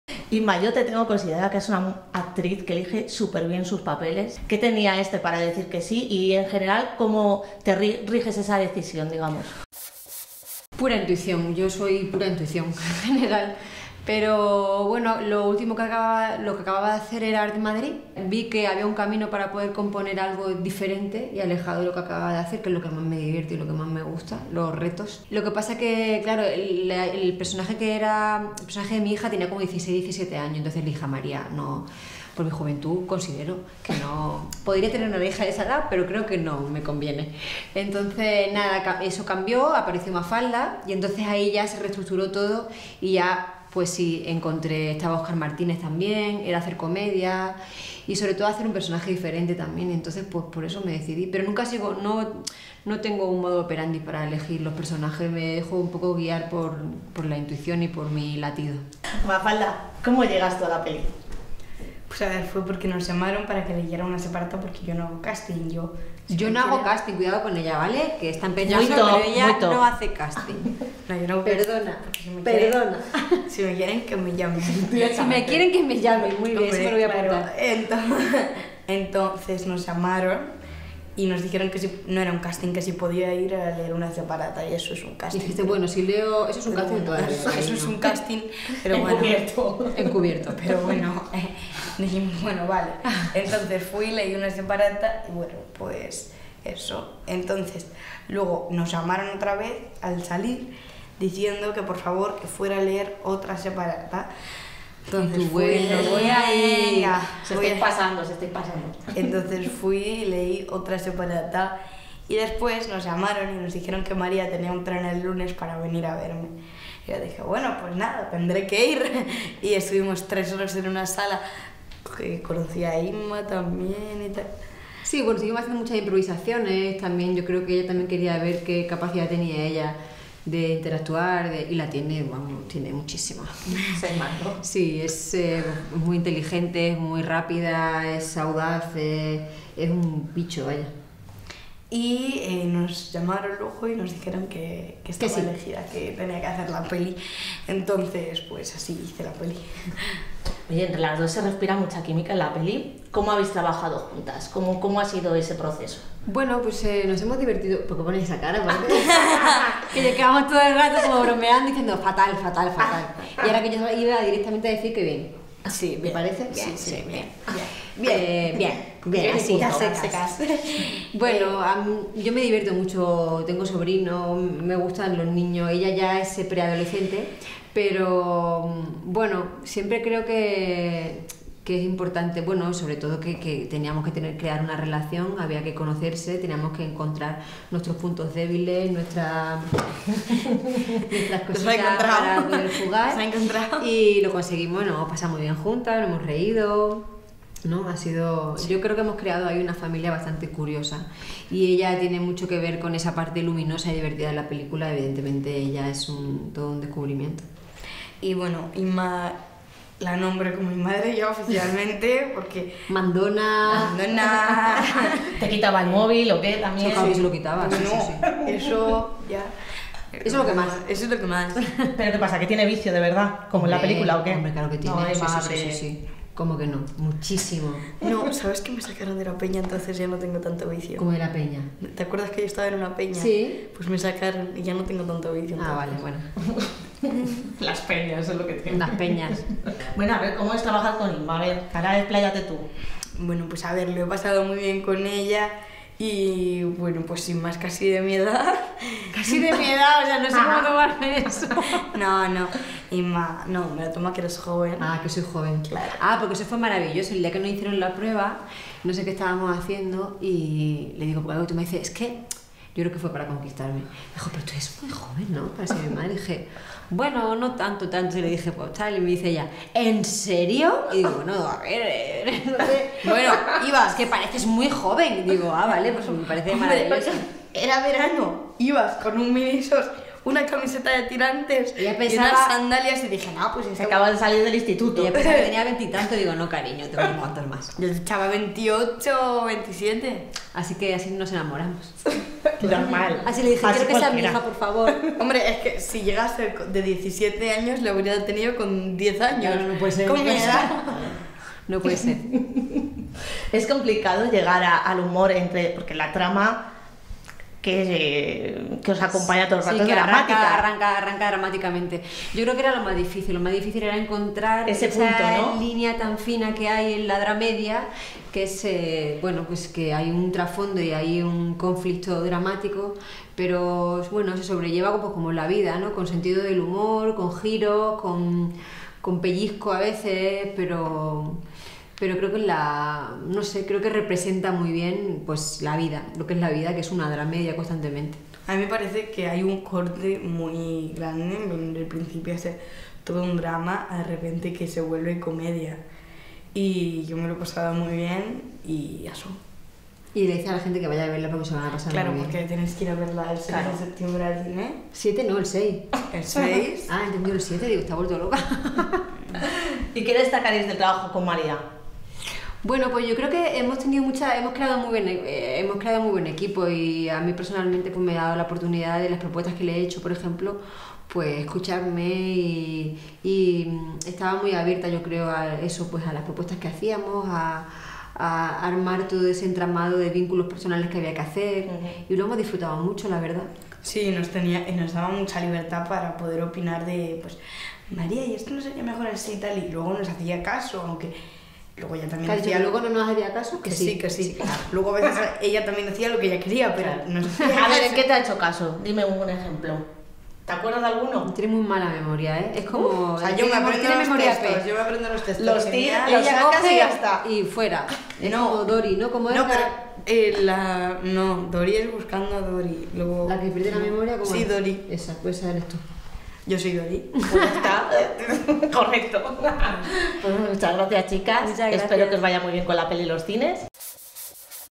Vivir dos veces: Inma Cuesta y Mafalda Carbonell
Entrevistamos a Inma Cuesta Y Mafalda Carbonell, madre e hija en la ficción y protagonistas de la película "Vivir dos veces".

Durante la entrevista de "Vivir dos veces" las protagonistas nos hablan sobre su experiencia en el rodaje. Mafalda nos cuenta cómo llegó a la película detallándonos su proceso de selección y afirma que ella "no hace castings". Inma Cuesta muestra su fantástica relación con la niña, haciendo las veces de madre durante el rodaje. De hecho, ambas nos cuentan que en más de una ocasión le quitó el móvil.